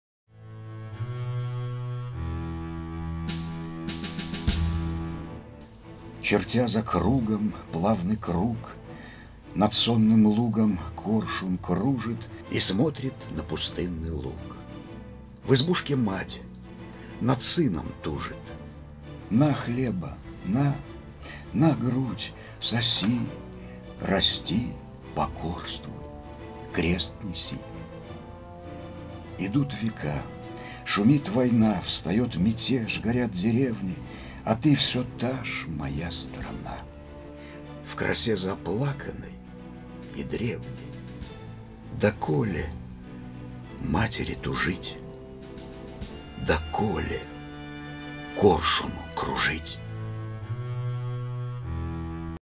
"Чертя за кругом плавный круг" 1916 год. Александр Блок
Читает Игорь Сидорчик
 © ЗАО «Второй национальный телеканал», 2015 год